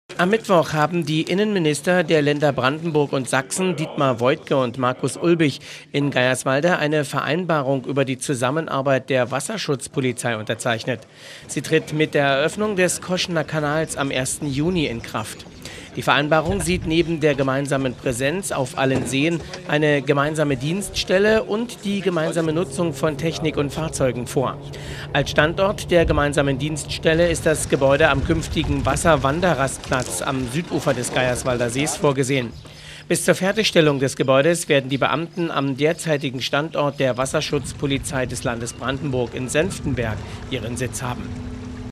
Unterschrift: Sachsen und Brandenburg mit gemeinsamer Wasserschutzpolizei - ELSTERWELLE
Am Mittwoch haben die Innenminister der Länder Brandenburg und Sachsen, Dietmar Woidke und Markus Ulbig, in Geierswalde eine Vereinbarung über die Zusammenarbeit der Wasserschutzpolizei unterzeichnet. Sie tritt mit der Eröffnung des Koschener Kanals am 1. Juni 2013 in Kraft. Die Vereinbarung sieht neben der gemeinsamen Präsenz auf allen Seen, eine gemeinsame Dienststelle und die gemeinsame Nutzung von Technik und Fahrzeugen vor. Als Standort der gemeinsamen Dienststelle ist das Gebäude des künftigen Wasserwanderrastplatzes am Südufer des Geierswalder Sees vorgesehen. Bis zur Fertigstellung des Gebäudes werden die Beamten am derzeitigen Standort der Wasserschutzpolizei des Landes Brandenburg in Senftenberg ihren Sitz haben.